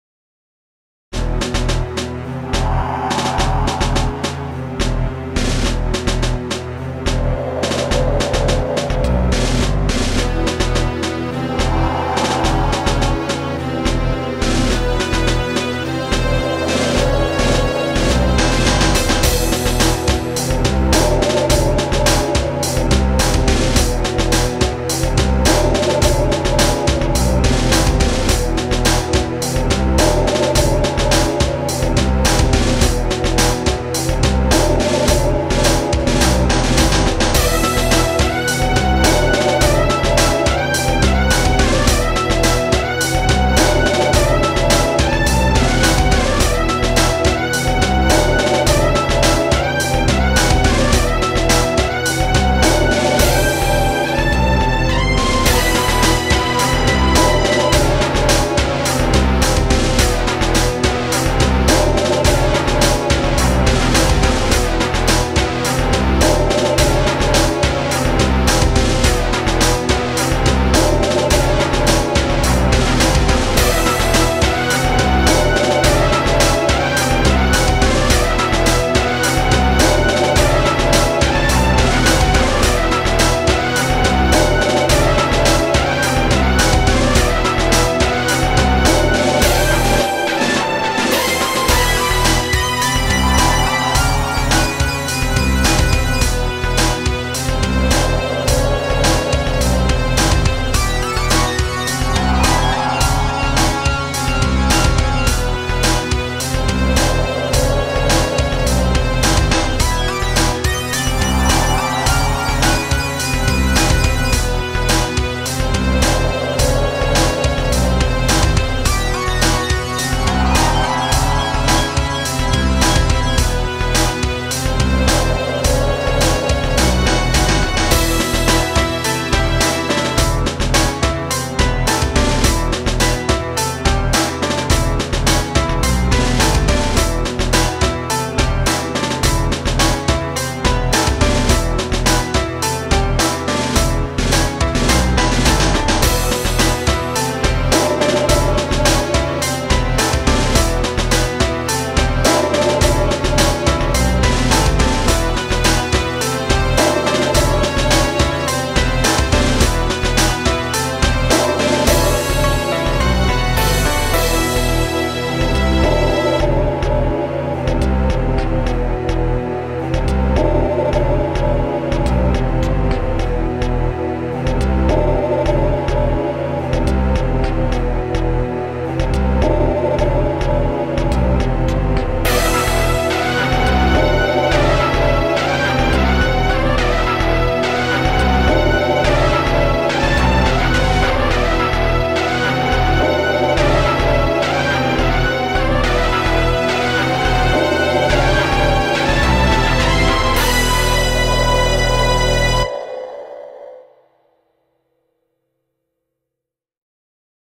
GoldenEye - Custom Track (2019)
Here is my latest custom track for GoldenEye on the N64, using the original instruments!